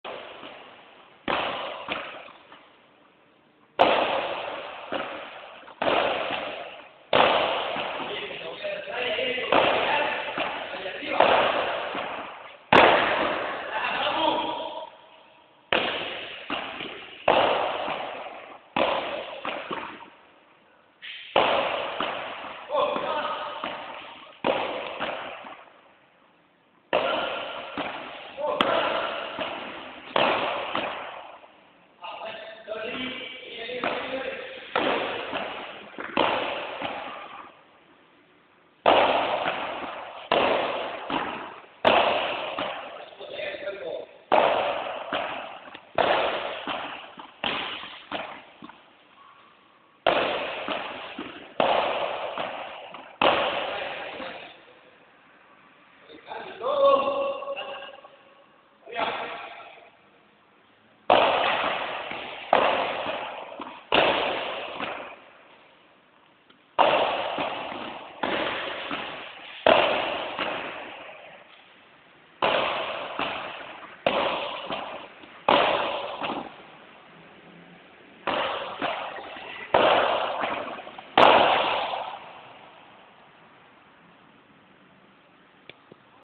Padel Andrés Moralez entrenamiento Smasch y Bolea
El "Enano" Andrés Moralez en pleno entrenamiento de Padel, práctica de Smash, volea de revés y de drive. El entrenador: Maxi Zárate, jugador Nº 1 de Misiones (Argentina). Cancha El Jardín (Posadas-Misiones).